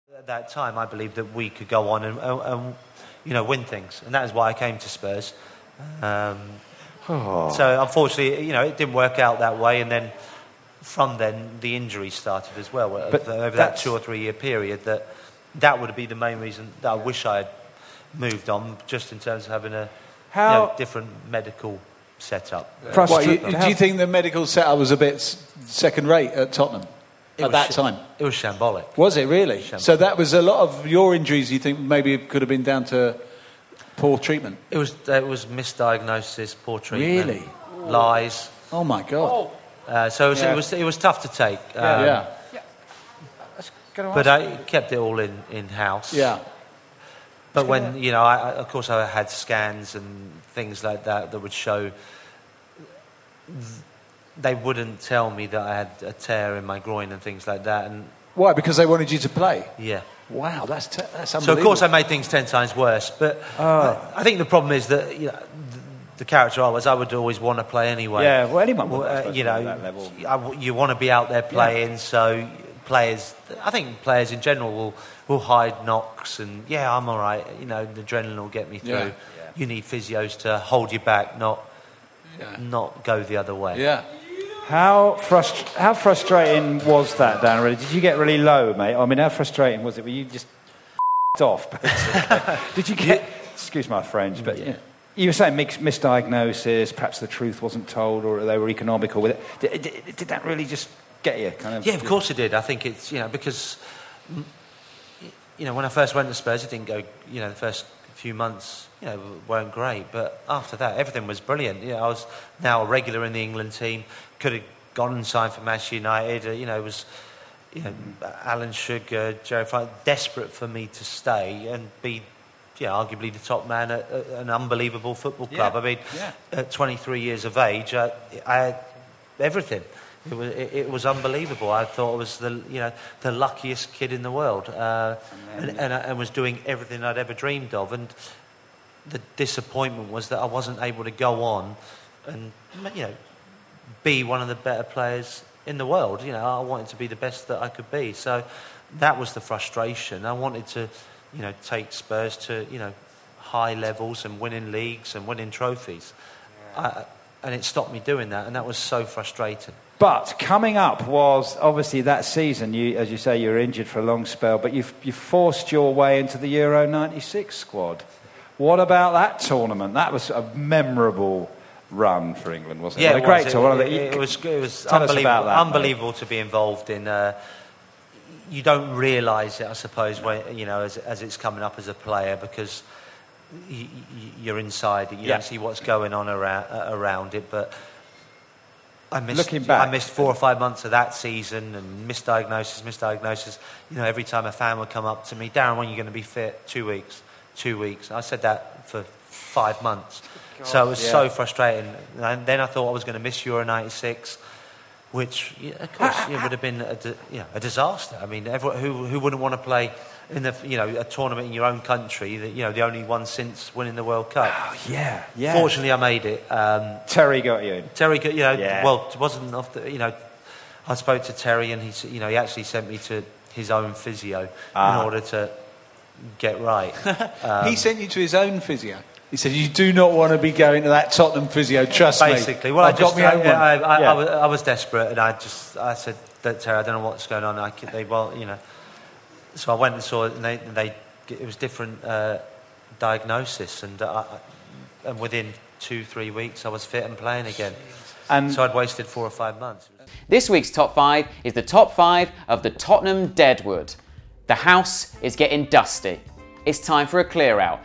"Spurs Medical Setup Was Shambolic!" | Darren Anderton At Spurs Show Live Podcast | Spurred On
At the Spurs Show Live Podcast recording, Darren Anderton ripped into Spurs' medical team.

Spurred On is the brand new Tottenham Hotspur Fan Channel made by Spurs fans, for Spurs fans.

Skype, Instagram, Snapchat, Vine, Meerkat, Periscope - Spurredontv